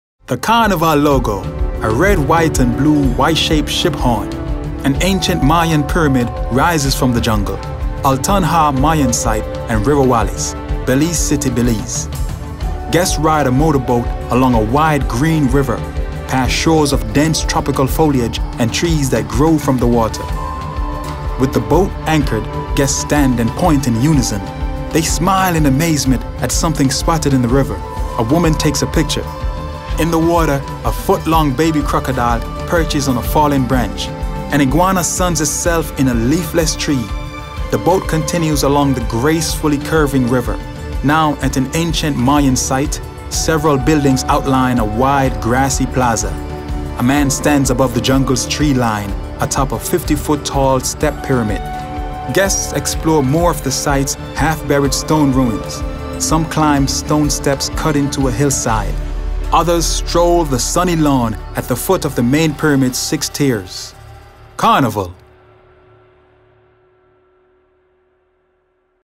Altun Ha Mayan Site & River Wallace in Belize | Carnival Cruise Line
Journey into Belize's wildlife in a lush tropical rainforest and visit important Mayan ruins on this fun shore excursion. Keep your eyes open on your way to the ancient Mayan ceremonial center as you just might spot crocodiles, iguanas, howler monkeys and more.

Arrive at the ancient Mayan center and explore the five square miles, three plazas and thirteen structures. Take breathtaking photos at one of the main temples - the panoramic view of the surrounding countryside is just marvelous. After this adventure, jump on the bus to head back to your fun ship.

Don't forget, there are plenty of things to do and see in Belize!